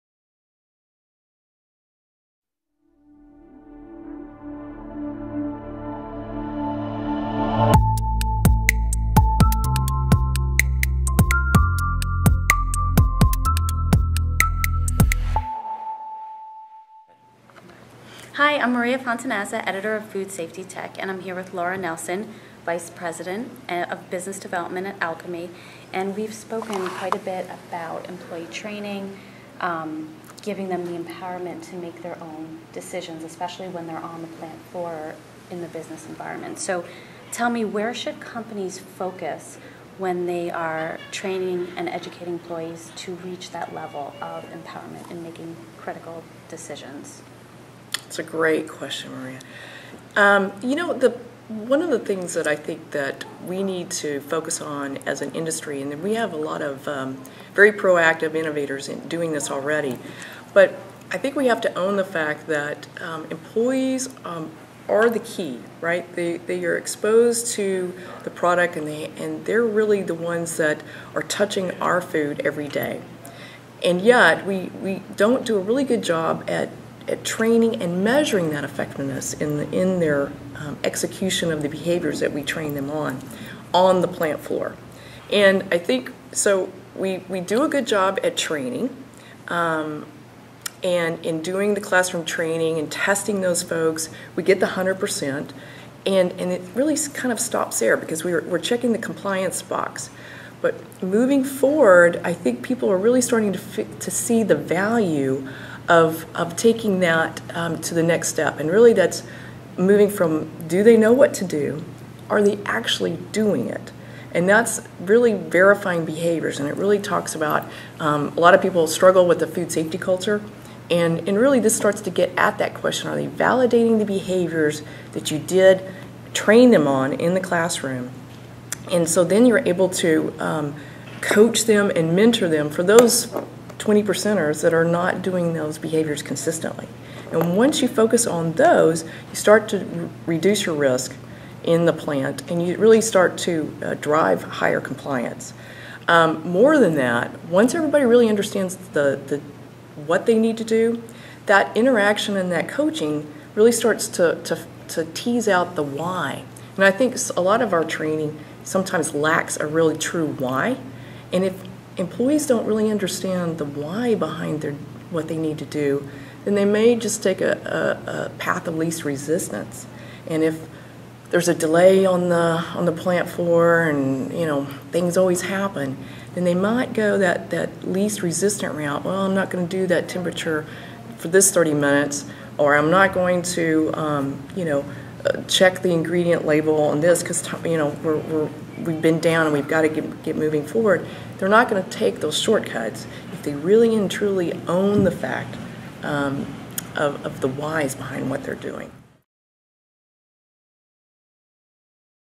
Empower Employees in Food Safety
In an interview with Maria Fontanazza of Food Safety Tech, Laura Nelson, vice president of business development at Alchemy, discusses the approach that food companies should take when training and educating employees to help them reach a level of empowerment to make critical decisions.